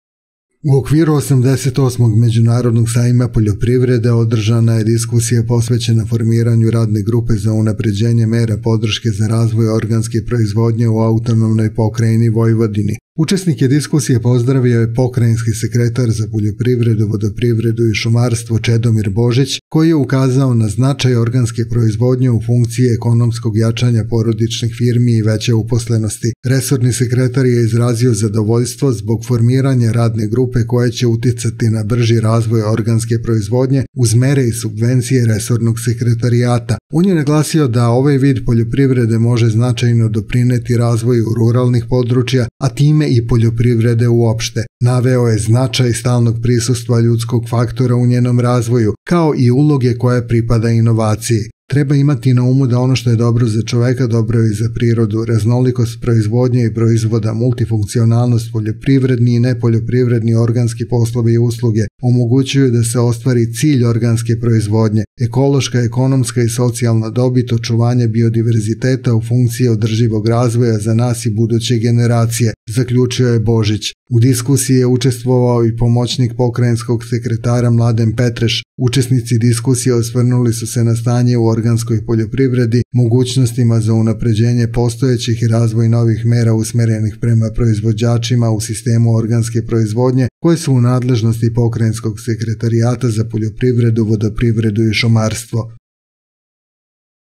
Vojvodina pomaže razvoj organske proizvodnje
Resorni pokrajinski sekretarijat odlučio je da posveti veću pažnju razvoju organske proizvodnje, koja je isplativa, zdrava i popularnija u celom svetu.